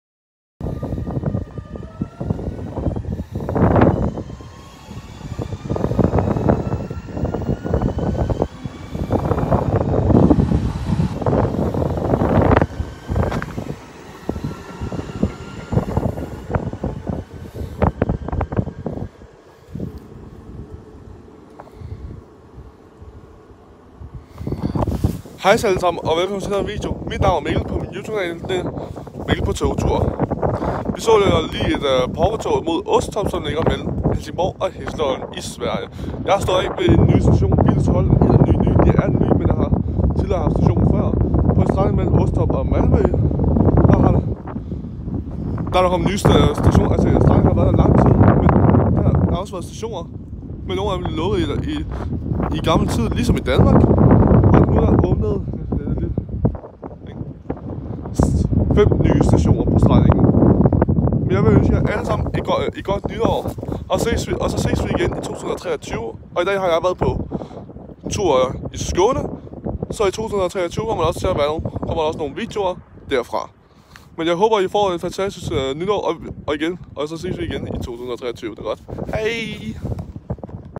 Godt Nytår fra Mikkel på Togtur
Håber at i kan høre mig men jeg håber også at i får et Godt Nytår, det siger jeg fra Billesholm i Skåne hvor vi ser et Pågatåg, endnu engang Godt Nytår.